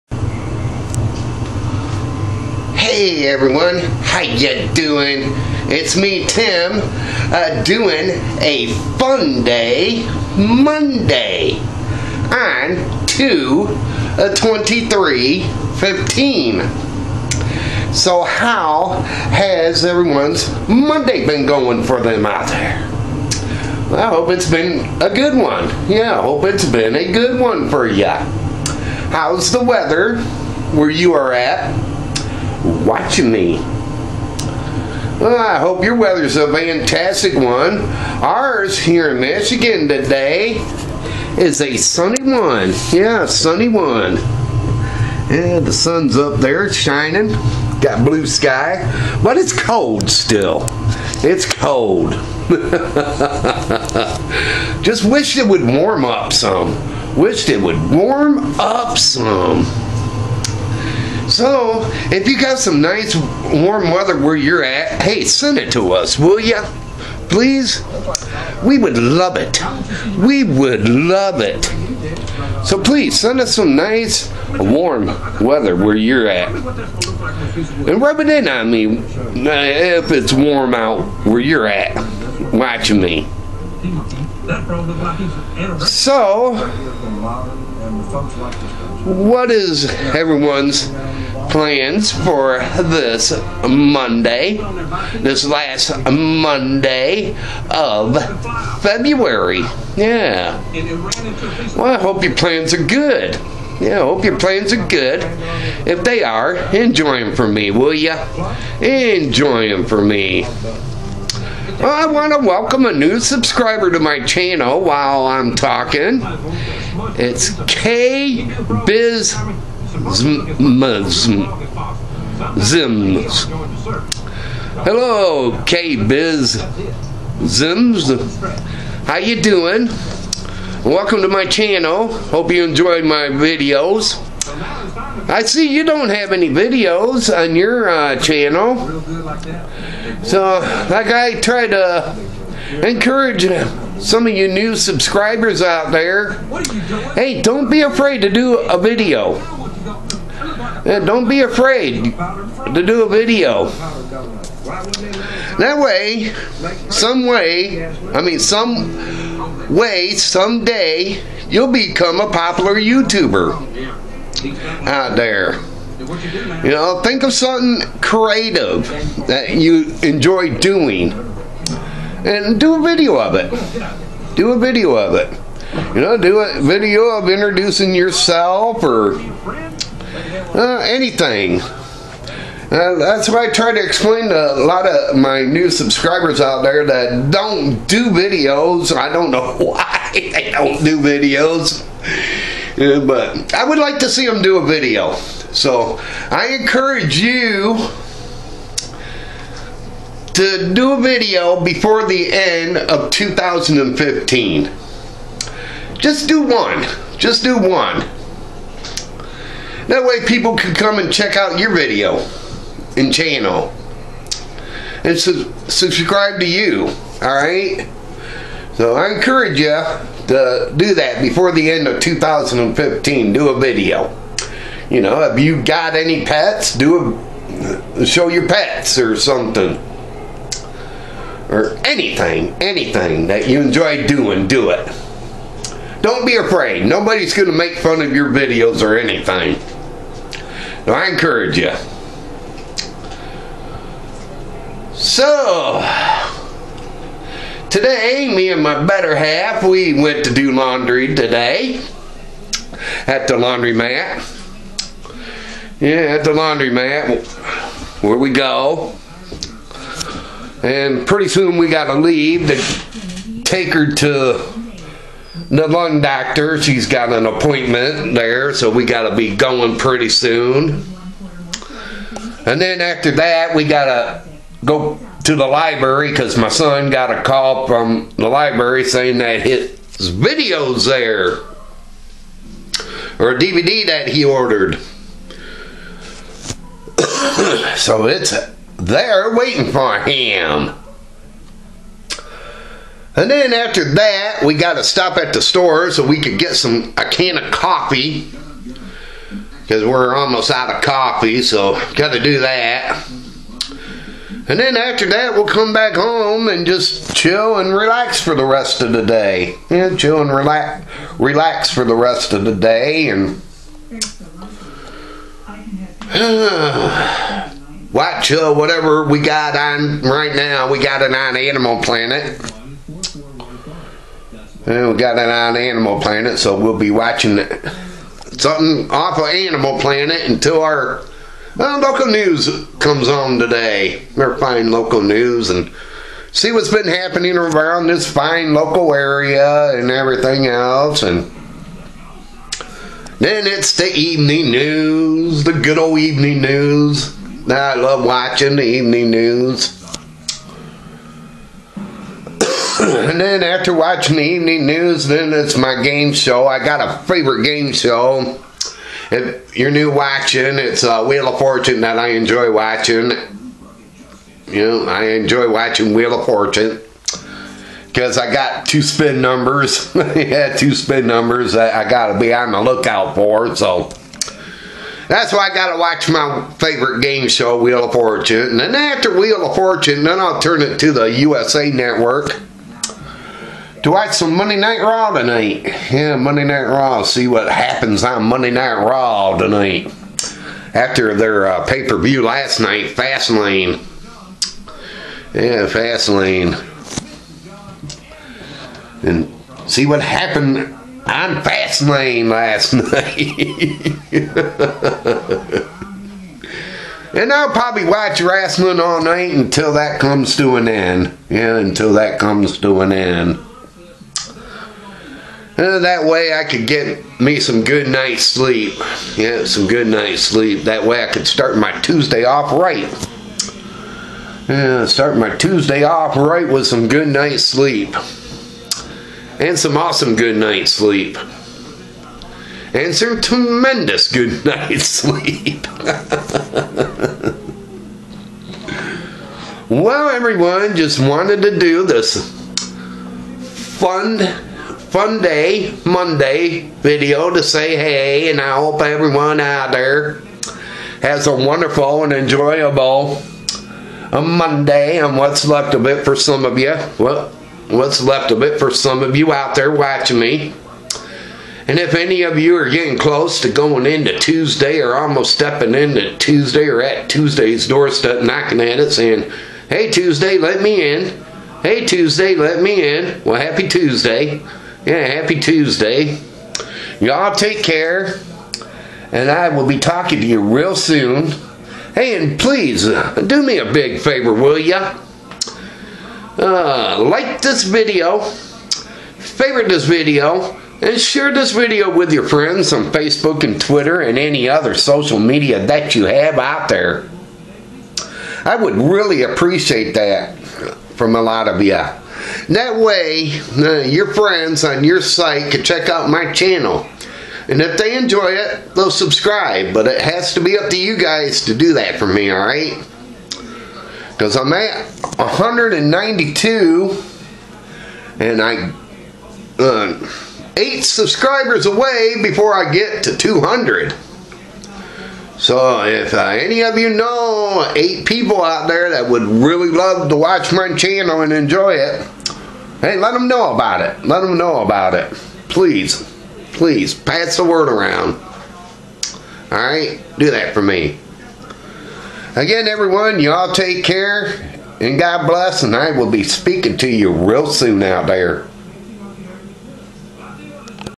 Funday Monday On 2/23/15
Talking And Showing What Are Weather Looks Like